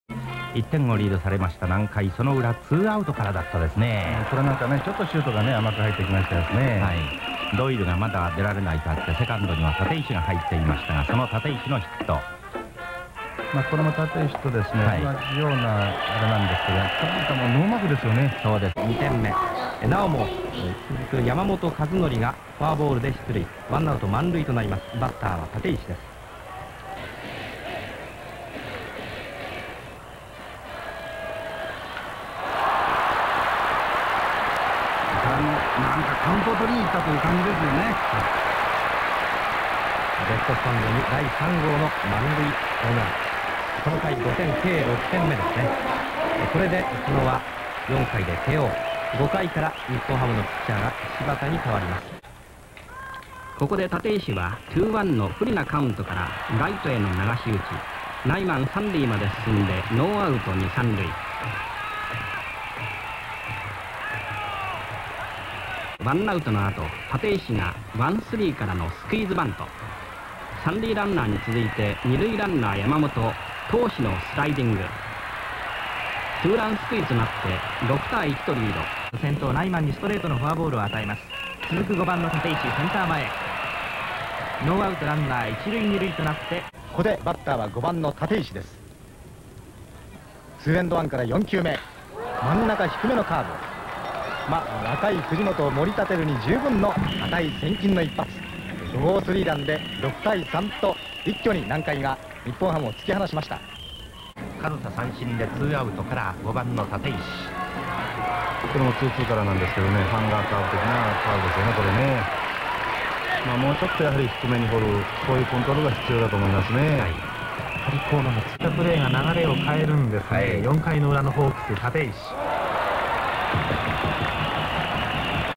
立石充男 ツーランスクイズ two-run squeeze～南海ホークスいぶし銀シリーズ～
隠し球で有名な立石です。
当時はまだ珍しい背番号ゼロ。
引退後も長年コーチを務められています。
Tateishi Mitsuo, Nankai Hawks.
two-run squeeze